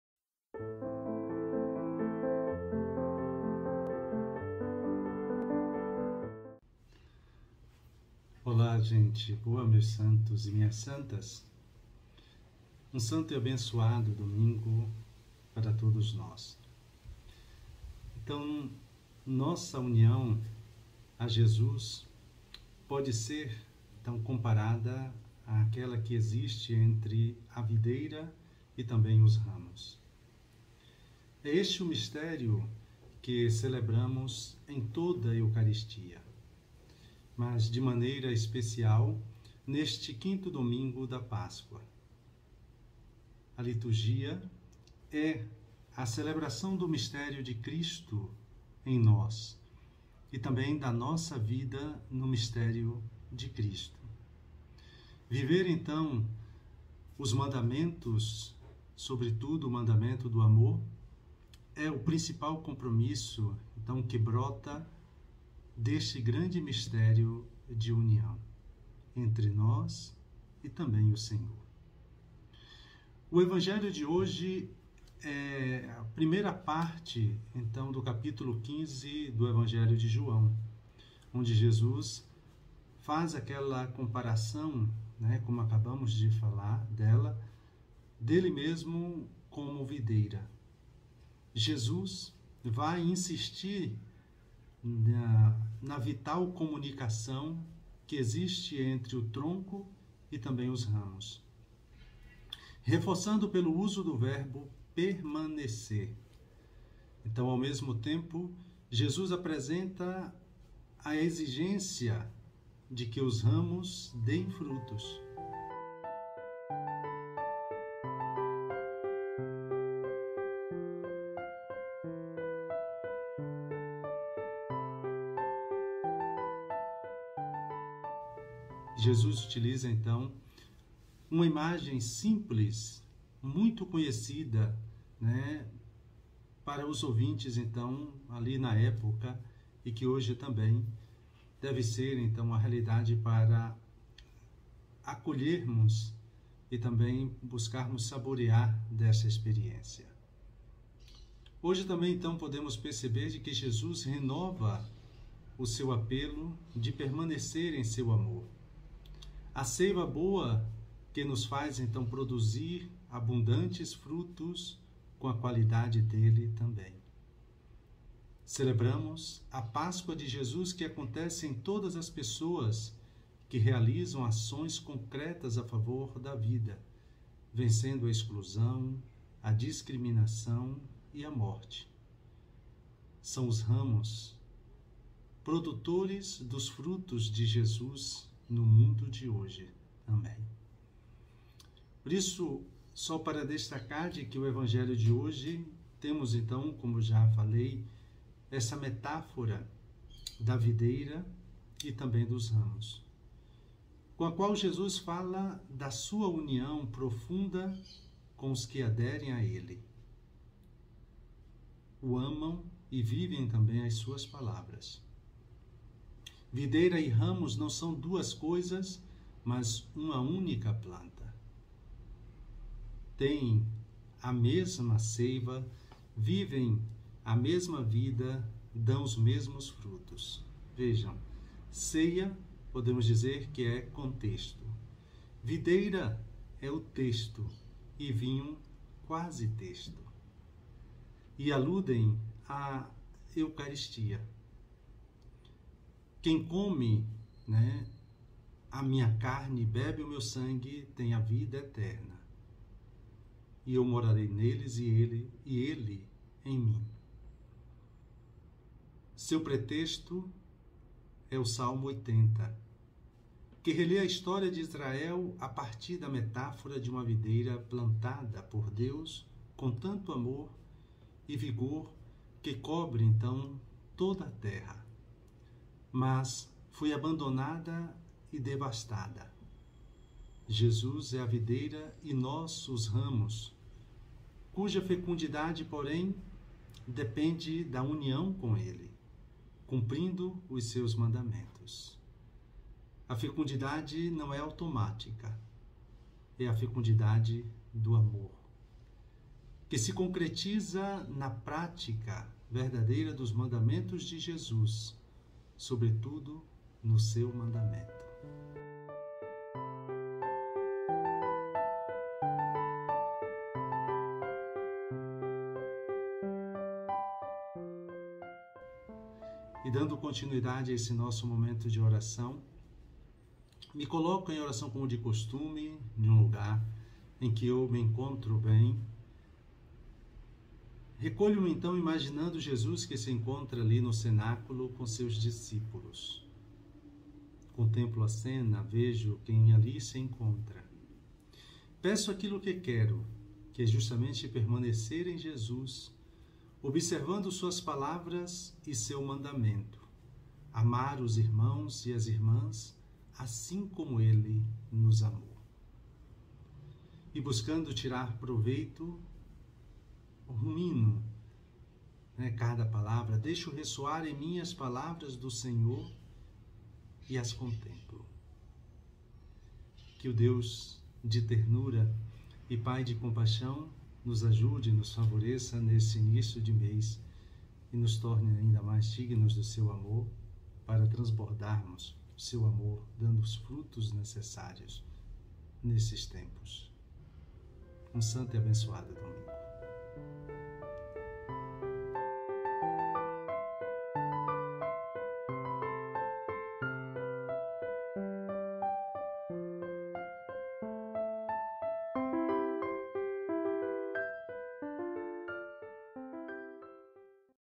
Momento de Oração - 2 de maio
Jesus é a videira verdadeira e nós devemos NELE permanecer para dar frutos. Assista à reflexão do Pe. Washington Paranhos SJ neste 5º Domingo da Páscoa.